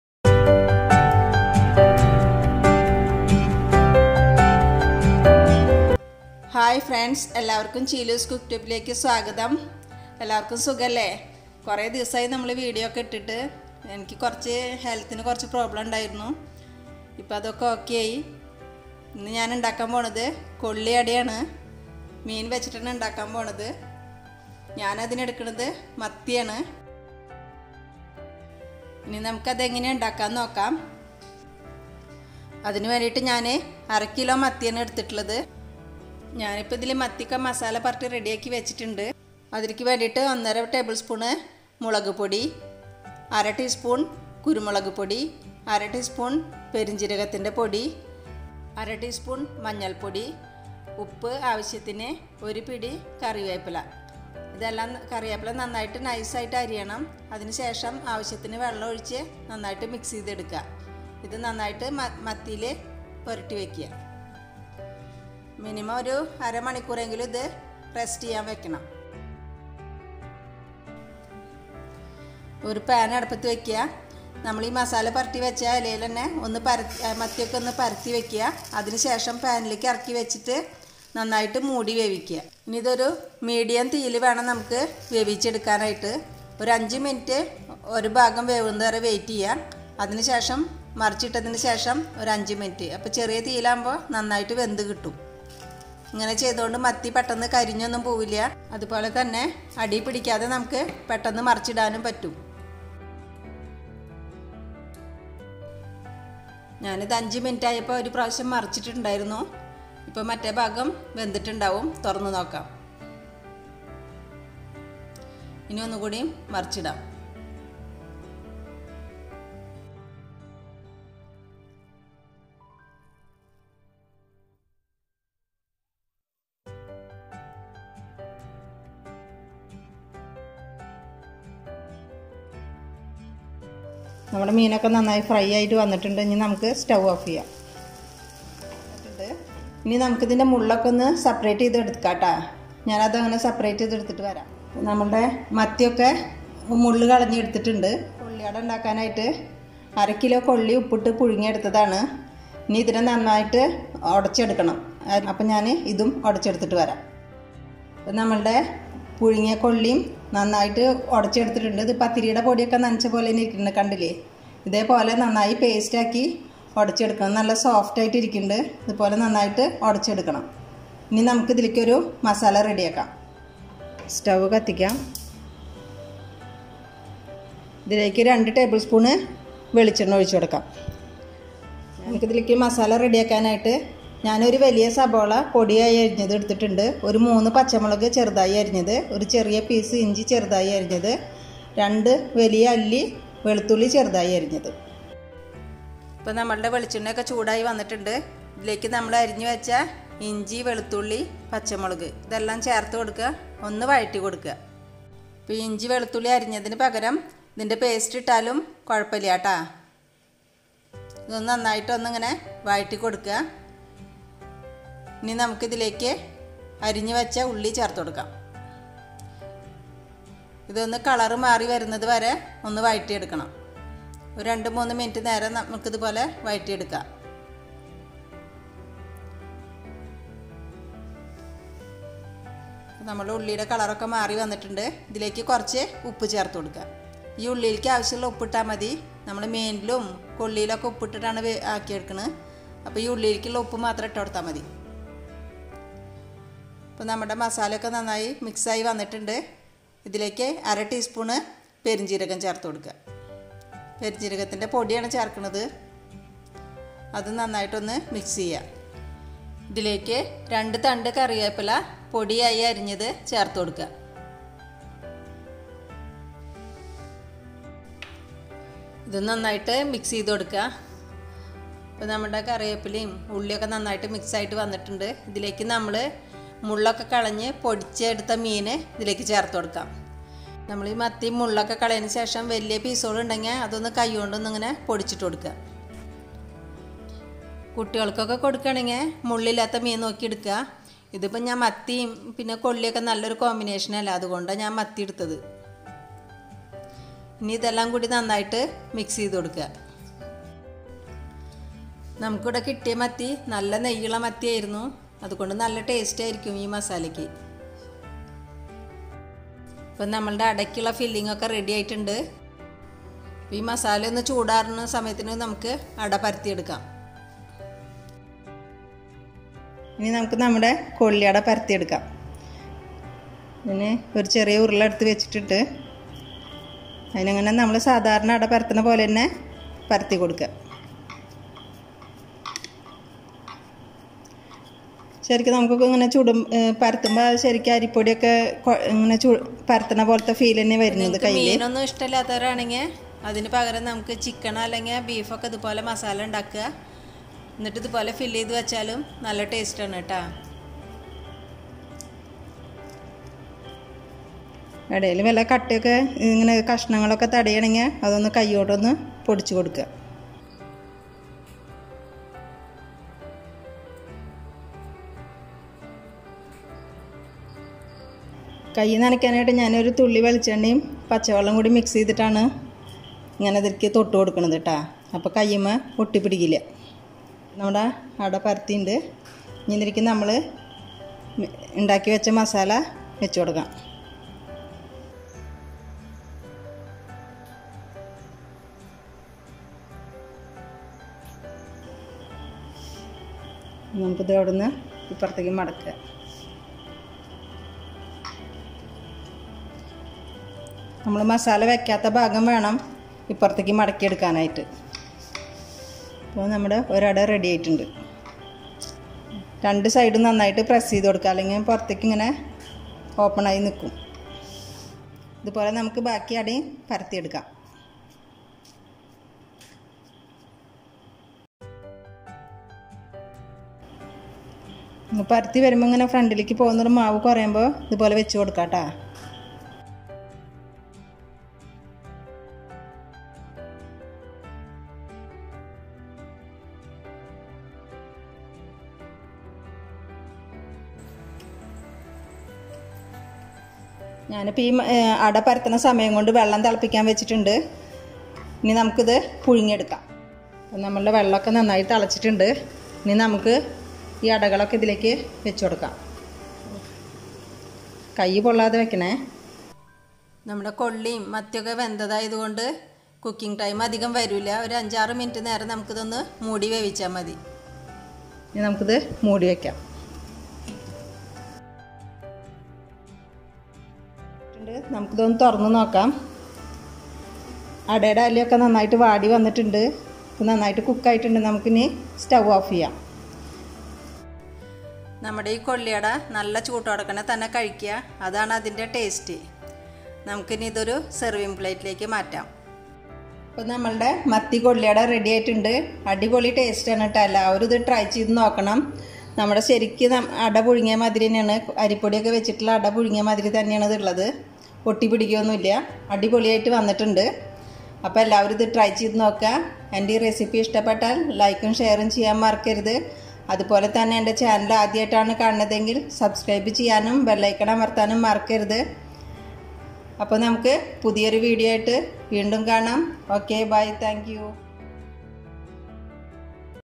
kolliada / kappa ada / ela ada / Tapioca ada recipe by cheelu's cooktube
Ingredients
1/2 kg  Kolli (Kappa/Tapioca)
1 Onion chopped (big)
3 Green chili
1 tbl spn Ginger chopped
1 tbl spn Garlic chopped
1/2 tsp fennel seed powder
1 tbl spn Curry leaves
2 tbl spn Coconut oil

Ingredients for sardine fry
1/2 kg Mathi (sardine)
1-1/2 tbl spn kashmiri chili powder
1/2 tsp Pepper powder
1/2 tsp Turmeric powder
1/2 tspFennel seed powder
2 tbl spn Curry leaves